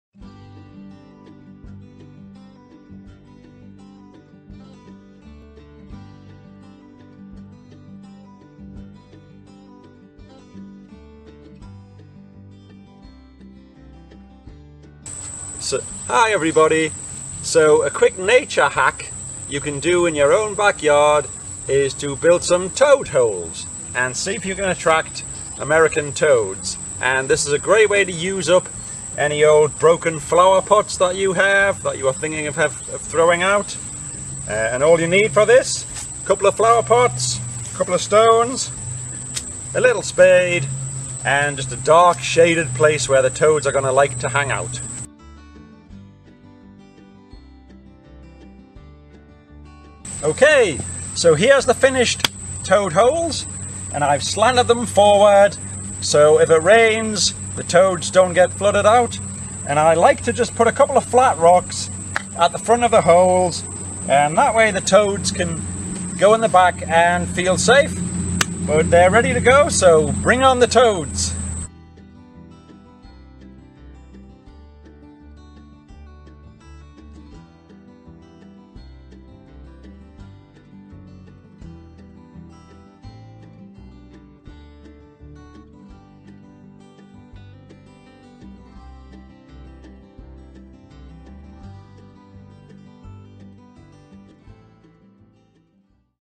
Nature Hacks - Toad Holes
Join Delaware Nature Society's Ornithologist Ian Stewart as he teaches little tricks to help make your yard wildlife friendly. Butterflies, birds, and other wildlife are in decline in part due to habitat loss and pesticides.  Do your part to help your local treasures!